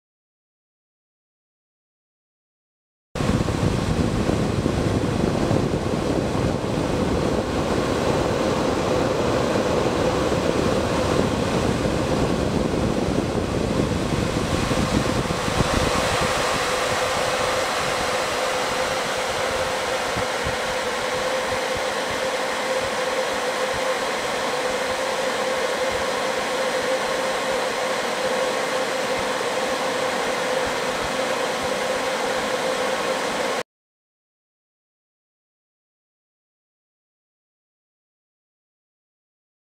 Closeup of a UV Air Freshener while working
A quick closeup of a small UV tube based air freshener device in operation. The UV tube changes Oxygen to Ozone, the Ozone freshens the air.